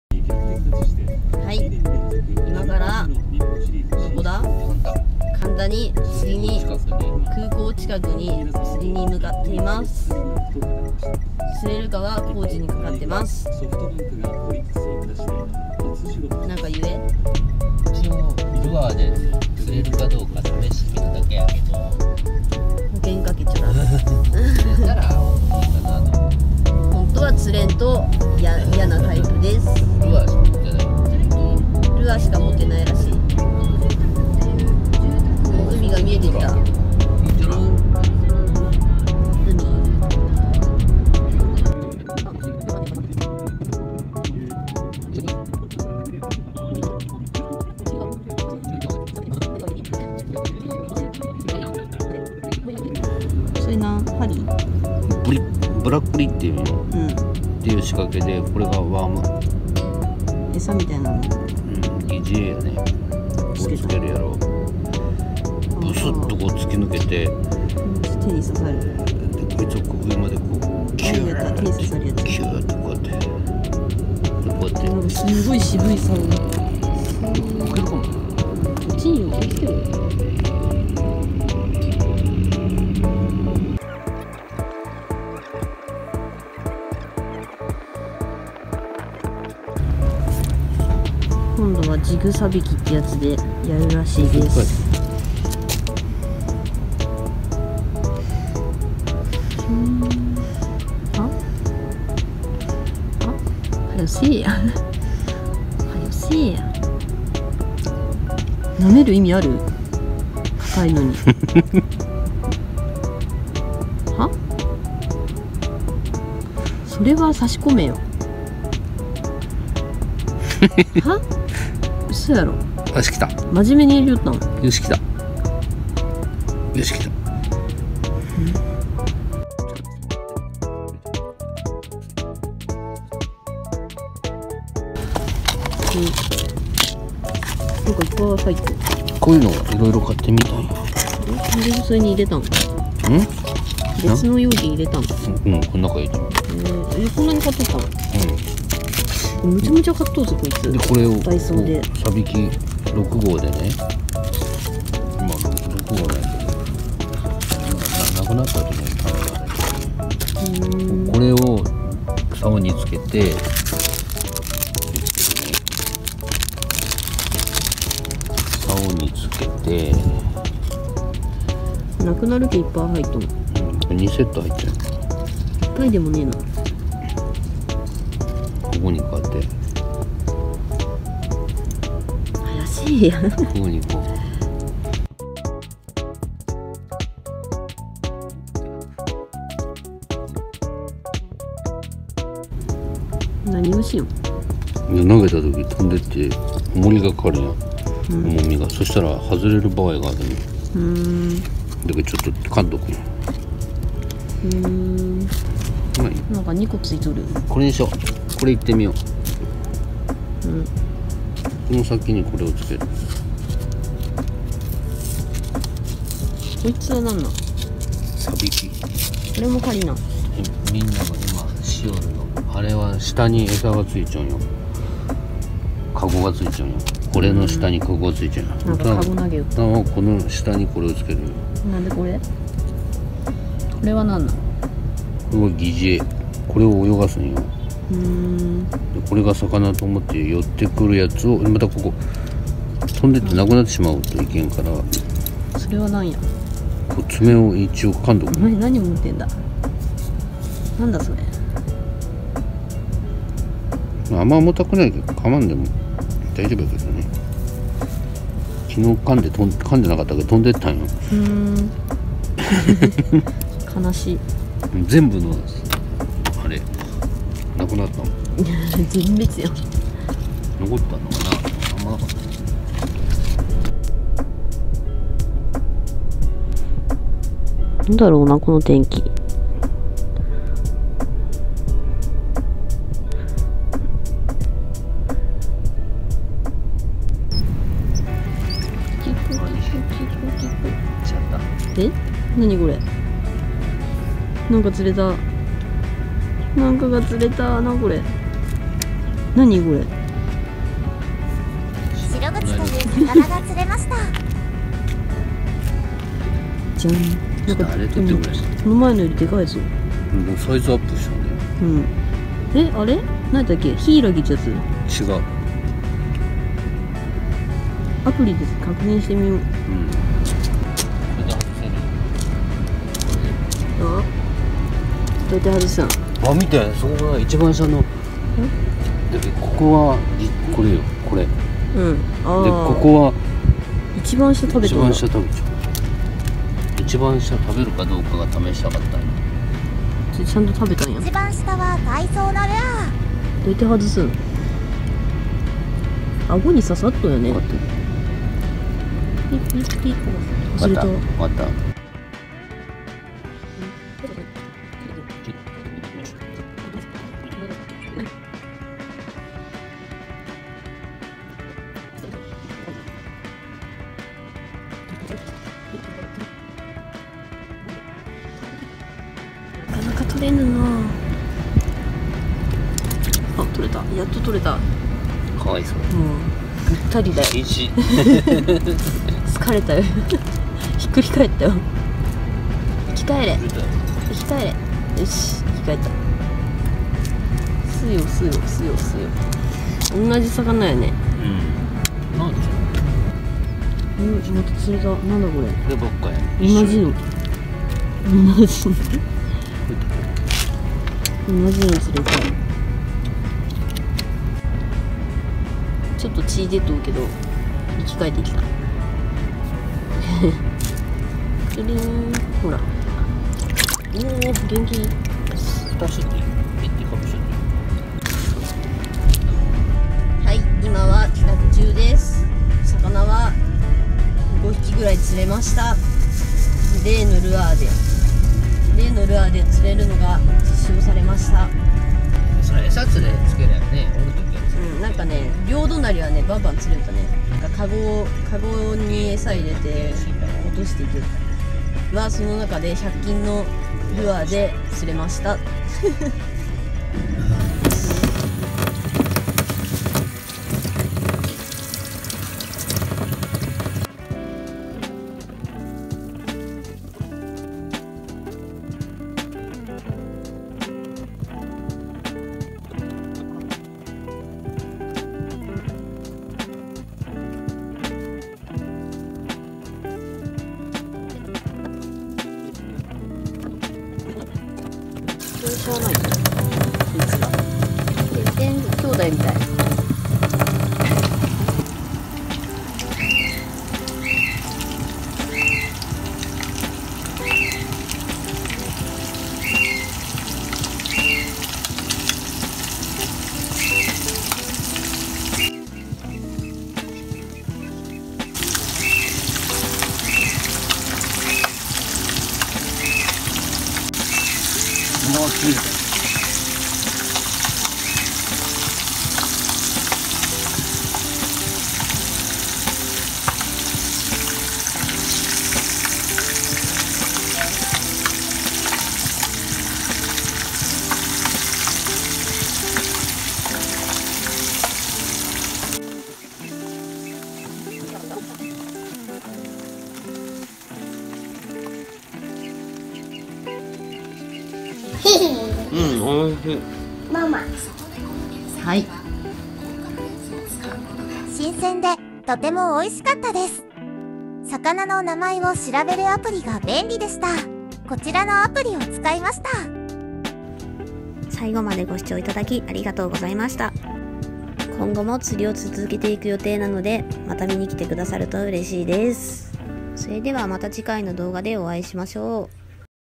【ダイソー釣具】ダイソーのルアーで魚は釣れるのか？
ダイソーで購入したルアーで釣りに挑戦！
果たして…⁉️

✨以下ネタバレ✨

シログチという魚が釣れました！👏👏👏
ダイソールアーでも十分釣れることが実証されました‼️
その他、ヒイラギというぬめぬめした魚も釣れました😆
さばいて調理して家族でいただきました🙏💖
また、フィッシュというアプリで魚判別していただきました！とても便利でありがたかったです😊✨